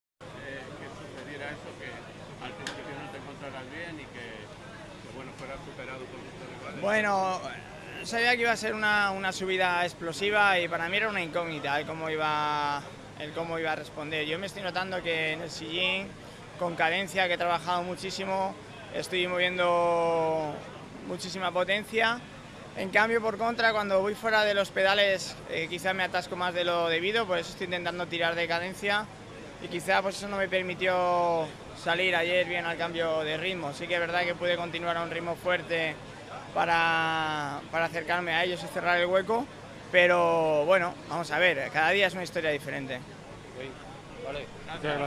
Declaraciones Alberto Contador ETAPA 6 TOUR DE FRANCIA 2017
Declaraciones de Alberto Contador en esta 6a Etapa del Tour de Francia 2017.